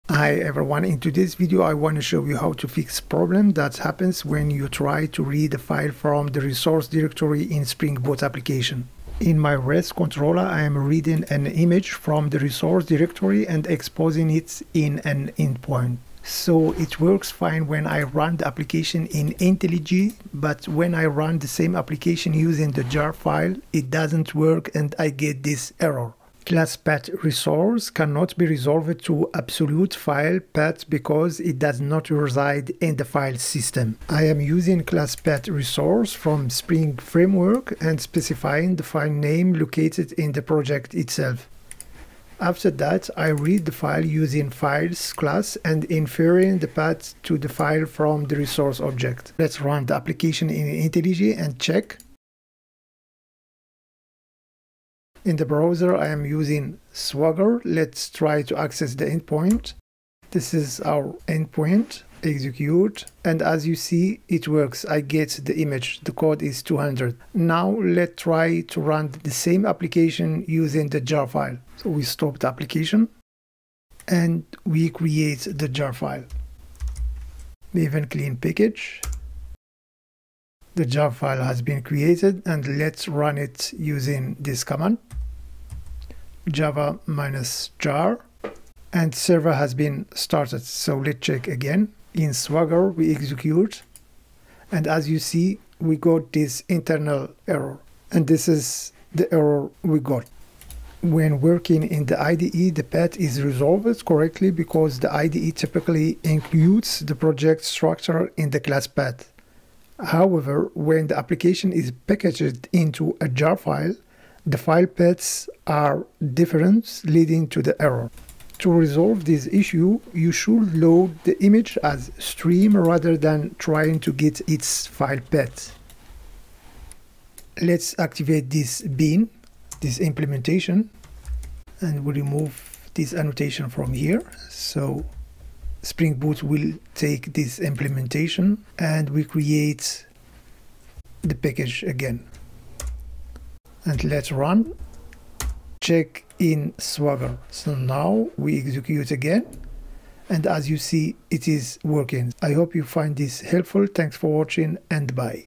Springboot How To Solve error class path resource filename cannot be resolved to absolute file path
I wonna show you how to fix a problem that happens when you try to read a file from the resource directory in a Spring Boot application.

When working in the IDE, the path is resolved correctly because the IDE typically includes the project structure in its classpath. However, when the application is packaged into a JAR file, the file paths are different, leading to the " error.